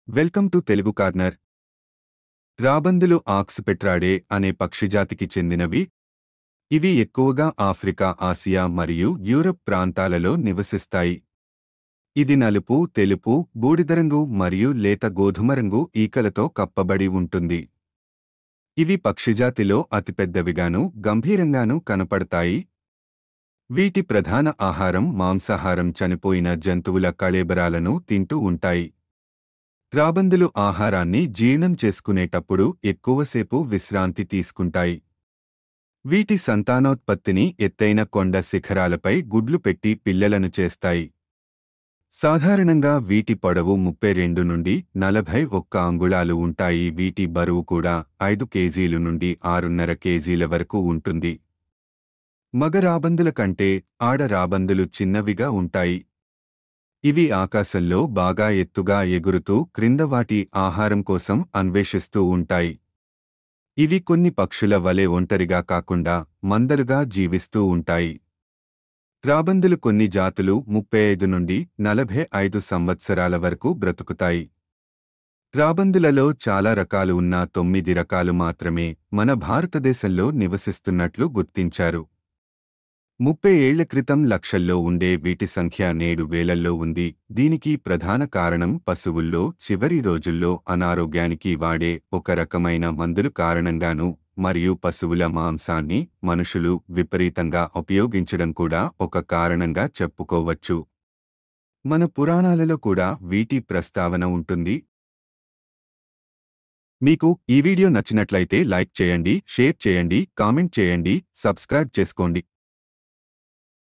About Vulture - రాబందులు గురించి - vulture bird in Telugu - Vulture videos - Vulture pakshi -
In this video describes about Vulture in Telugu. know moreover some facts about Vulture

ఈ వీడియోలో రాబందులు  గురించి తెలుగులో కొన్ని విషయాలు వివరించబడ్డాయి.